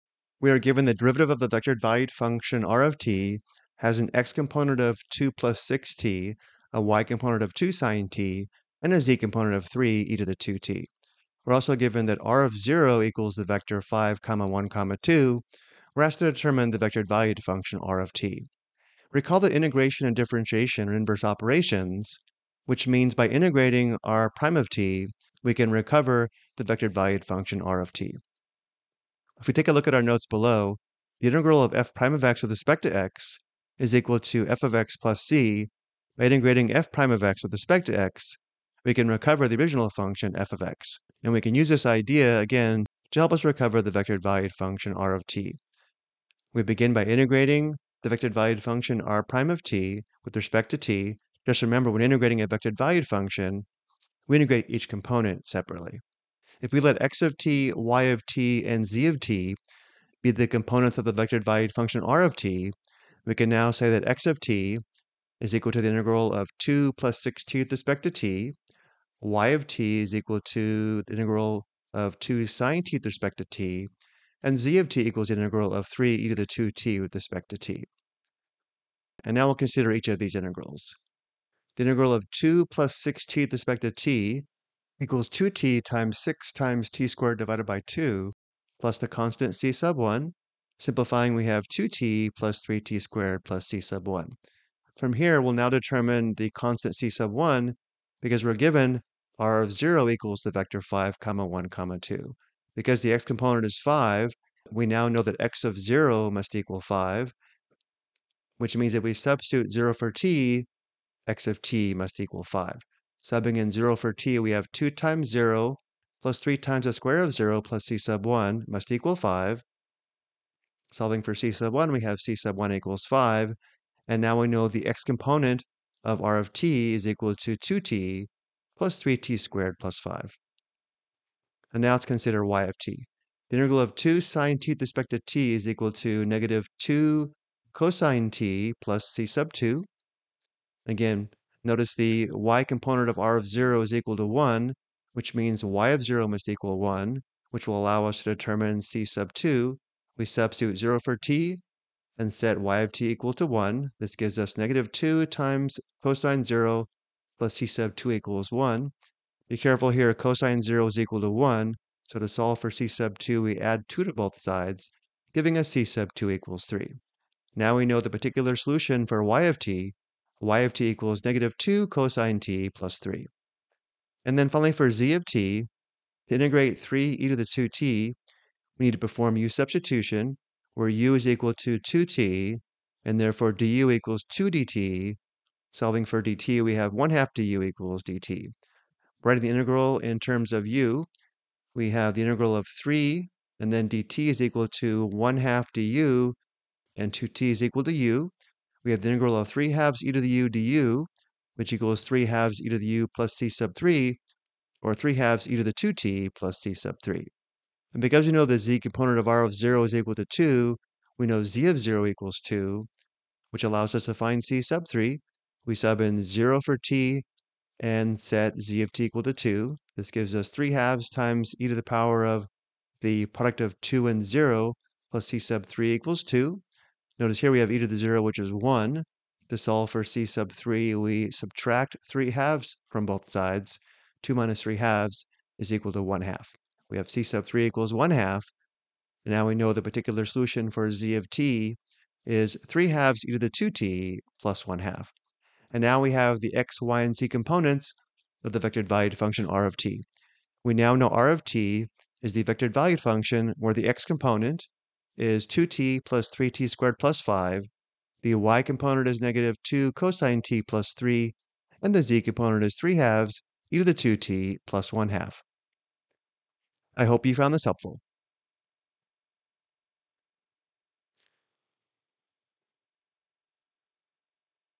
Determine a Vector Valued Function From Its Derivative: Integration Application
This video explains how to recover a vector valued function from its derivative using integration.